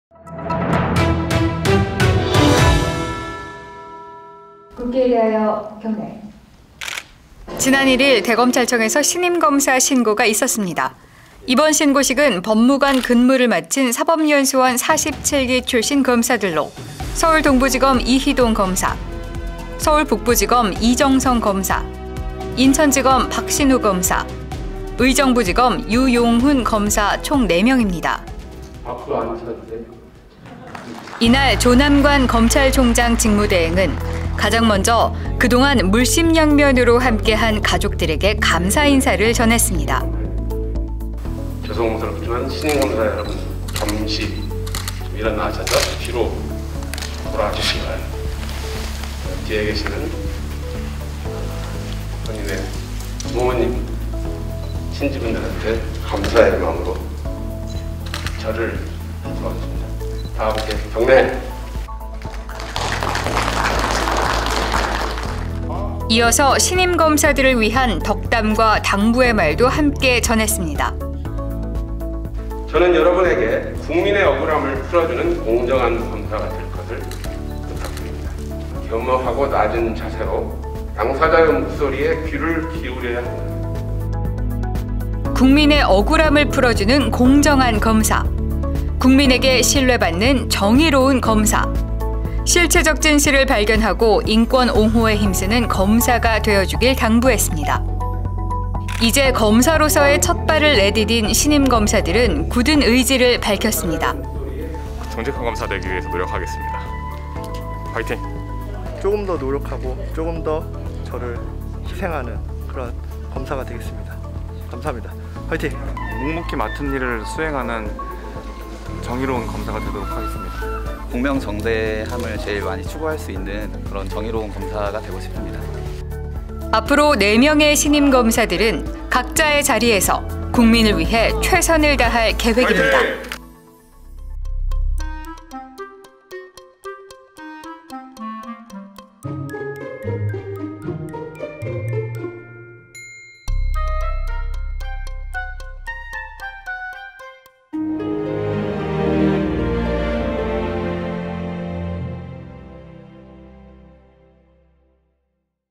2021년 신임검사 신고식
2021년 신임 루키 검사들의 신고식이 진행됐습니다!

정의로운 검사, 국민을 이해하는 검사를 꿈꾸며 오랜 시간 공부하고 준비해온 총 4명의 신임 검사들.

검사로서 새로운 첫 발걸음을 내디딘 그들을 지금 만나보세요!